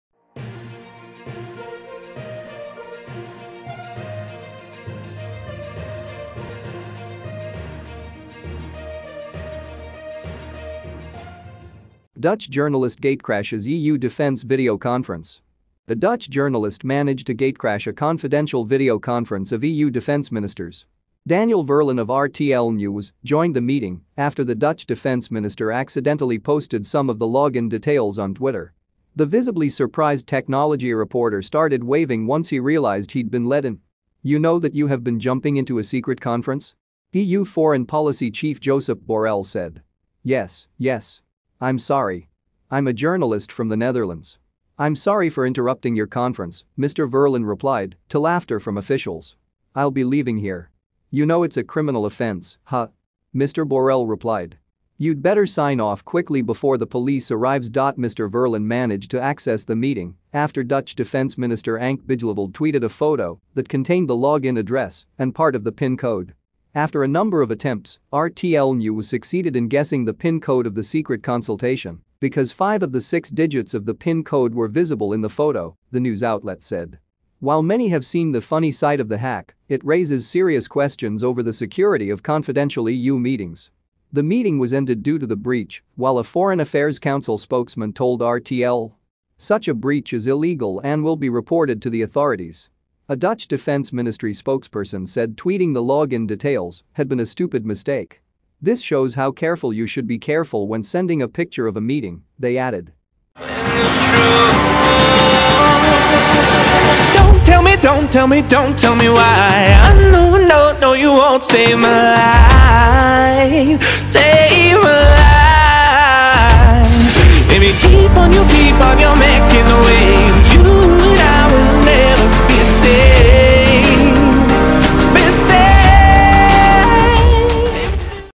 Dutch journalist gatecrashes EU defence video conference
A Dutch journalist managed to gatecrash a confidential video conference of EU defence ministers.

Daniel Verlaan of RTL Nieuws joined the meeting after the Dutch defence minister accidentally posted some of the login details on Twitter.

The visibly surprised technology reporter started waving once he realised he'd been let in.

"You know that you have been jumping into a secret conference?" EU foreign policy chief Josep Borrell said.

"Yes, yes. I'm sorry. I'm a journalist from the Netherlands. I'm sorry for interrupting your conference," Mr Verlaan replied, to laughter from officials. "I'll be leaving here."

"You know it's a criminal offence, huh?" Mr Borrell replied. "You'd better sign off quickly before the police arrives."Mr Verlaan managed to access the meeting after Dutch Defence Minister Ank Bijleveld tweeted a photo that contained the login address and part of the PIN code.

"After a number of attempts, RTL Nieuws succeeded in guessing the PIN code of the secret consultation, because five of the six digits of the pin code were visible in the photo," the news outlet said.

While many have seen the funny side of the hack, it raises serious questions over the security of confidential EU meetings.

The meeting was ended due to the breach, while a Foreign Affairs Council spokesman told RTL: "Such a breach is illegal and will be reported to the authorities."

A Dutch defence ministry spokesperson said tweeting the login details had been a "stupid mistake".

"This shows how careful you should be careful when sending a picture of a meeting," they added.